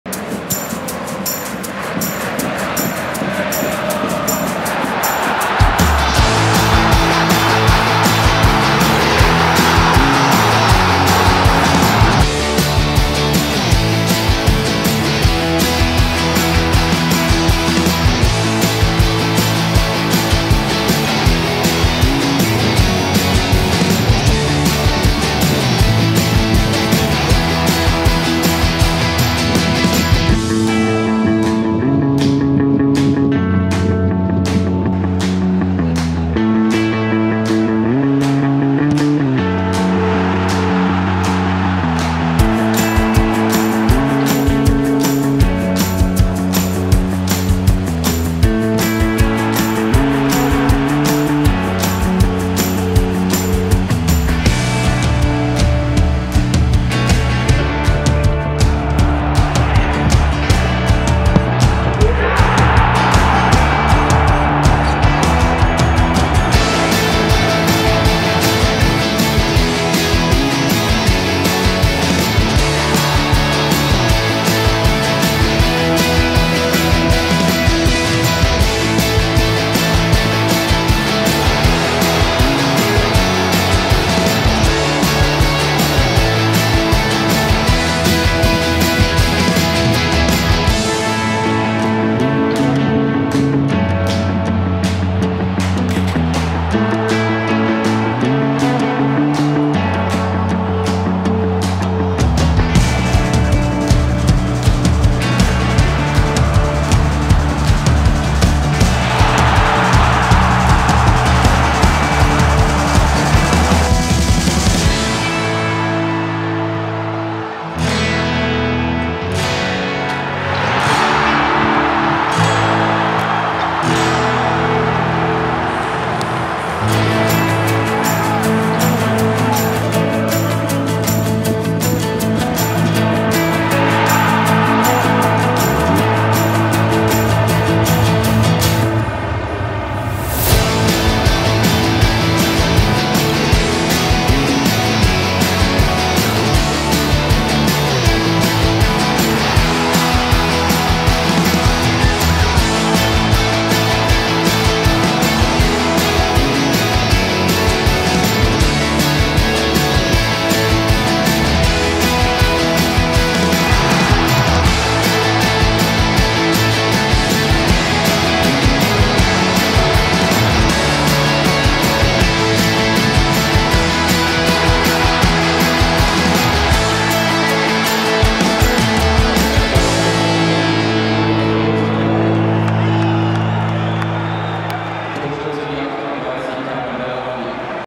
Best of Karim Bellarabi – Seine schönsten Tore im Trikot von Bayer 04 Leverkusen
Happy Birthday, Karim Bellarabi! 🎈
Seit 2011 steht Karim Bellarabi bei Bayer 04 Leverkusen unter Vertrag. In über 280 Pflichtspielen für die Werkself gelangen unserer Nummer 38 knapp 60 Treffer. Anlässlich seines 32. Geburtstags schauen wir zurück auf über ein Jahrzehnt unterm Bayer-Kreuz, genauer gesagt auf schöne Treffer. Mögen noch viele weitere hinzukommen...